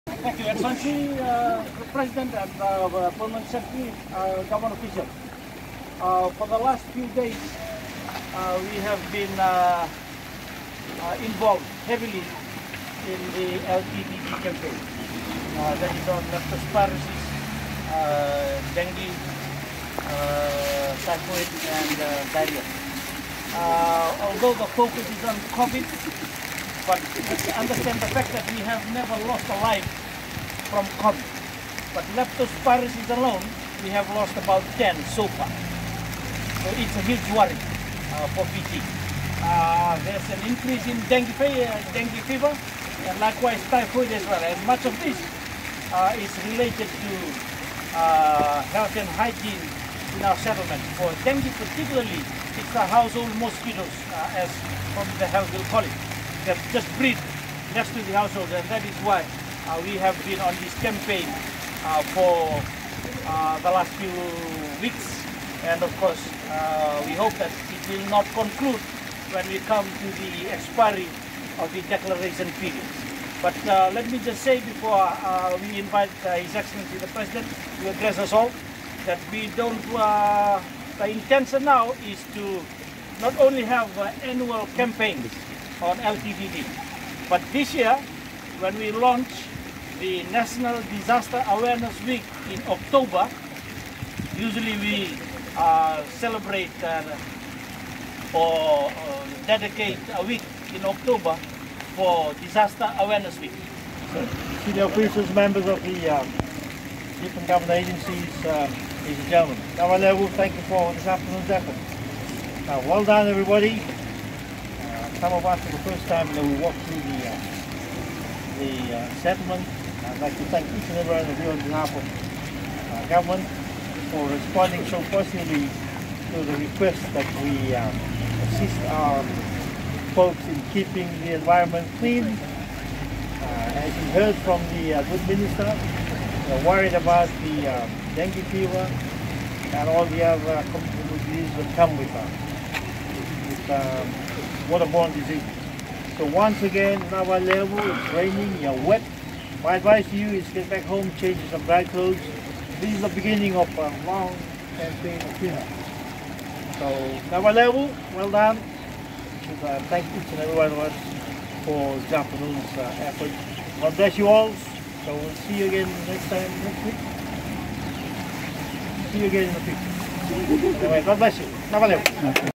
LTDD Campaign
Residents in the informal settlements of Nanuku and Veidogo in Vatuwaqa were surprised as they watched President Jioji Konrote lead the charge to clean up the area yesterday.

Civil servants including Minister for Defence, National Security and Policing, Rural and Maritime Development and Disaster Management Inia Seruiratu were part of the team that took on the task to clean up the Vatuwaqa area.

This was part of the LTDD – Leptospirosis, Typhoid, Dengue and Diarrhea Clean Up Campaign, which has been organised to ensure a stop on other communicable diseases as the nation fights the COVID-19.

“I thank everyone for this effort. It was a job well done,” said President Konrote as he spoke to the civil servants.

He got into the thick of things as he cleared tyres and containers which collected water.

“For the last few days we have been involved heavily in the LTDD Campaign. Although the focus is on COVID, the fact remains that we have never lost a life from COVID, leptospirosis alone, we have lost 10 so far,” Mr Seruiratu said.

“It is a huge worry for Fiji, there is an increase in dengue and typhoid as well. Much of this is related to health and hygiene.”

The campaign was targeted at the informal settlements in Vatuwaqa and Jittu Estate in Samabula. The informal settlements have been recorded to have such cases with health hygiene being a problem.